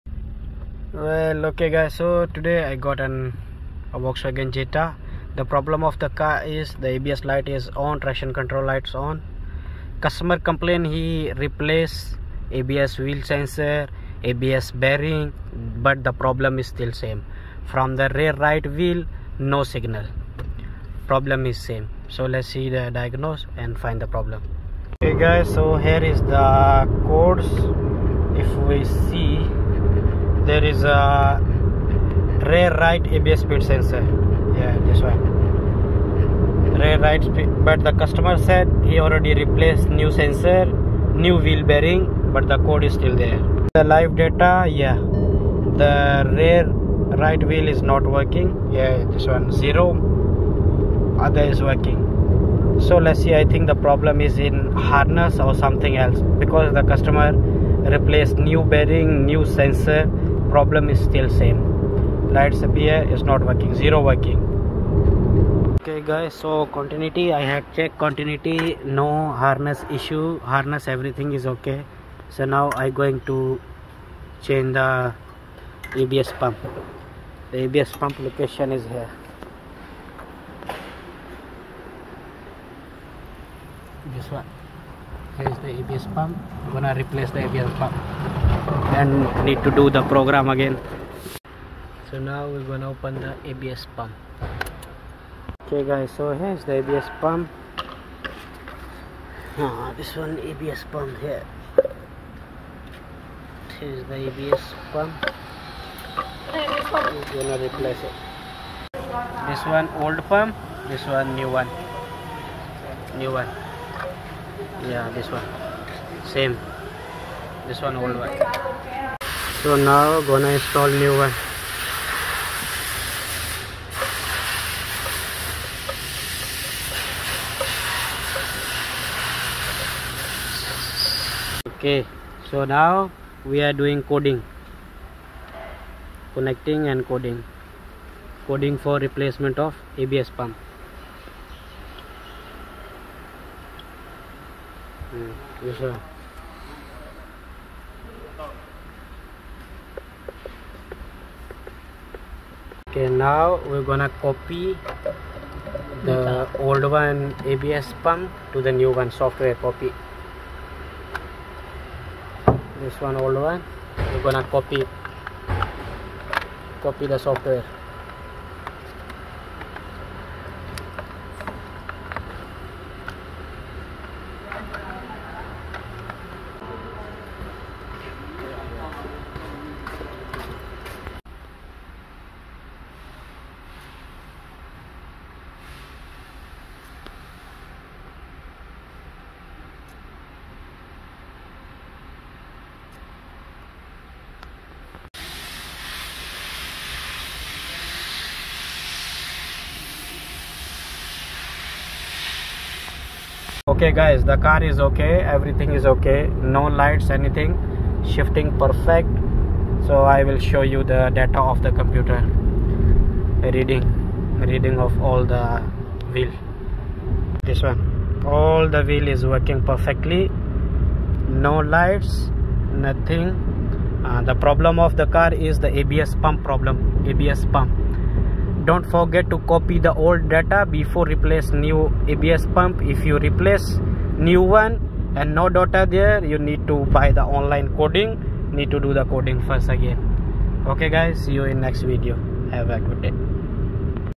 U101700 P1887 00287 Mechanical Failure Abs lights on Abs Control Module Impulseable signal
Hi there in this video we gonna learn how to fixed the Abs lights on and traction control lights on when you drive above 30 Km and lights are pop up for a Audi and volkswagen vehicles here is the basic you need to check and find the issue

1 vehicle Speed sensor faulty
2 vehicle Wheel Bearing Hub
3 Harness short to ground or open circuit or high circuit
4 fuse for Abs Pump
5 live data from Diagnosis tool
6 voltage
7 Continuity on vehicle Harness for speed sensor all Harness for speed sensors coming from Abs pump  directly
8 Abs pump faulty

in my case customer replace new speed sensor and new wheel bearing but code can't earse so I find the that Abs pump module not giving proper signal to speed sensor which is Rear Right sensor after replace secand hand Abs pump vehicle all 4 speed sensors working perfectly and all code gone after replace new parts you need to drive atleast above 30Km for sensor getting signal